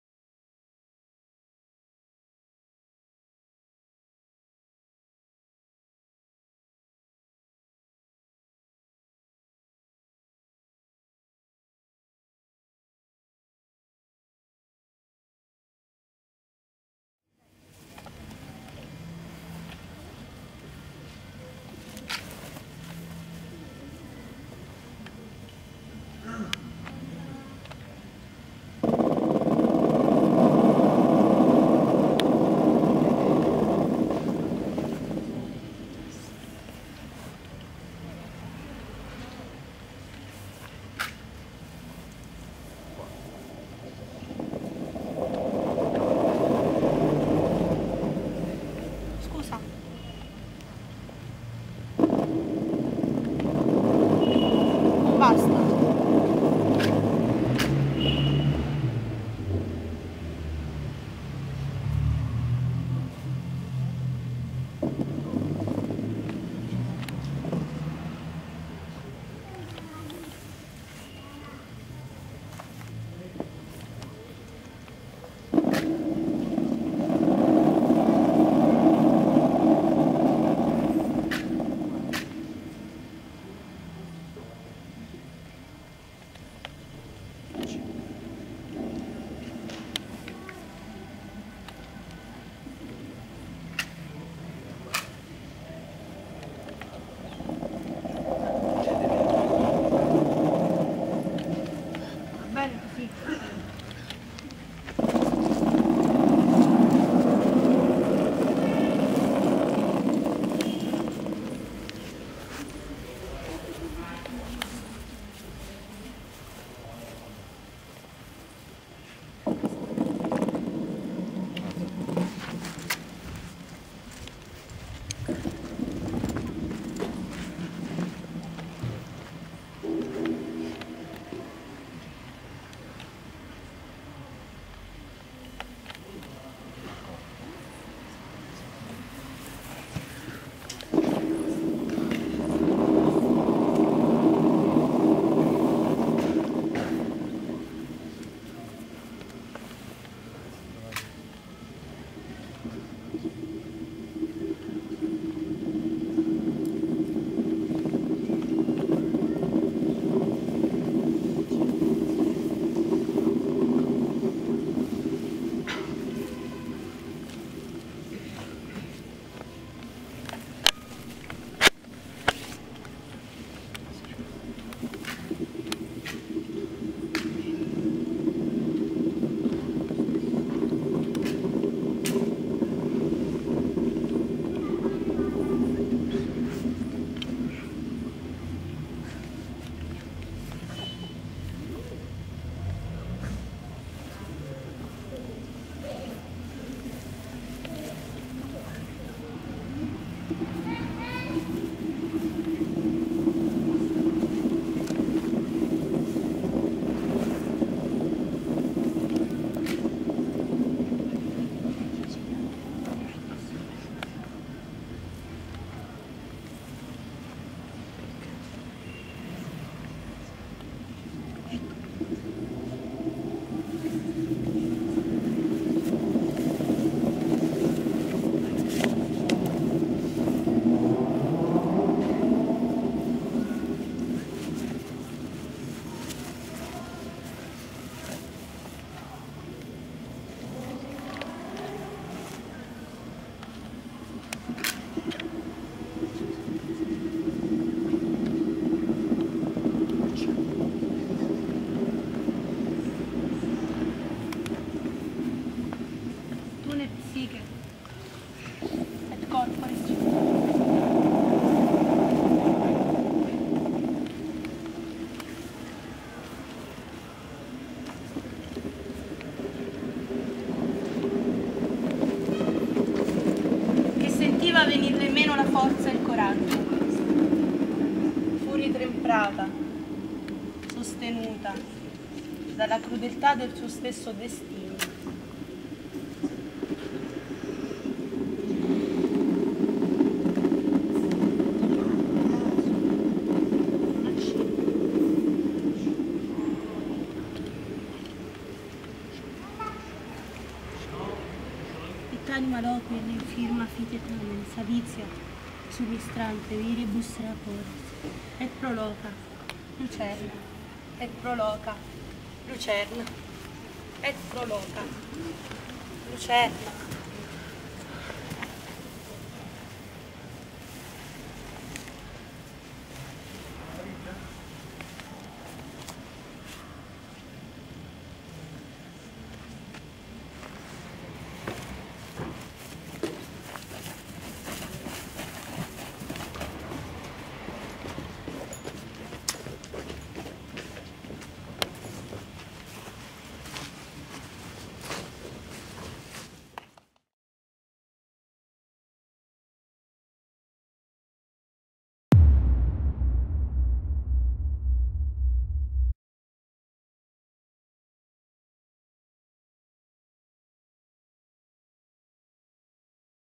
AWOP 2009 Duo Di Martino/Rescigno - parte II
Gli artisti Di Martino/Rescigno si esibiscono nei giardini della Biblioteca Comunale di Villaricca.
A World of Peace 2009 - Villaricca, 5 Luglio 2009